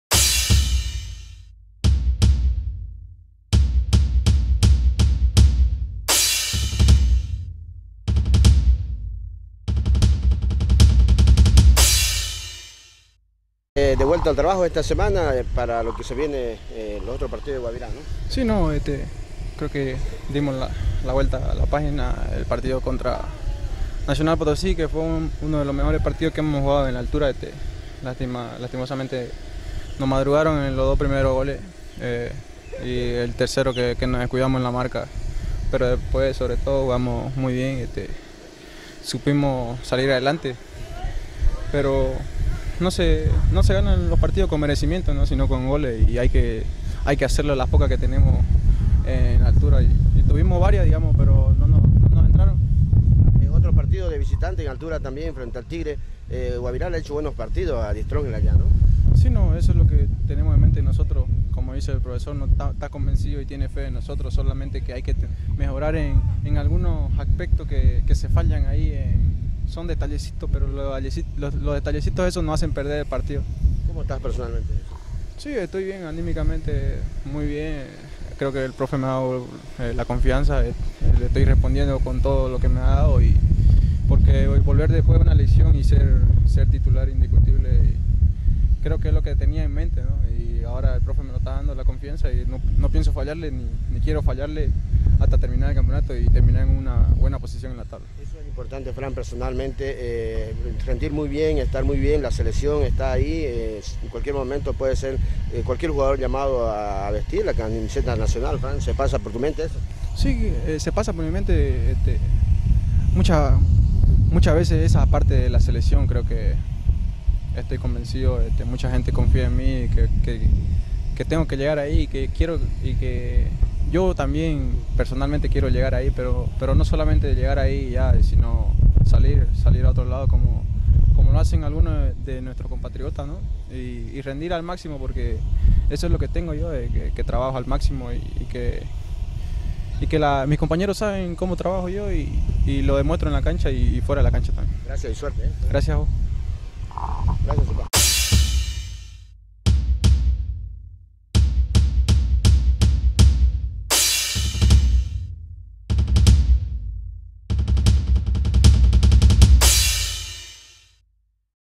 SUPAYABE FRANK
En FURIA ROJA el defensor Rojo habla  de la fecha 15 del torneo clausura de la División del Fútbol Boliviano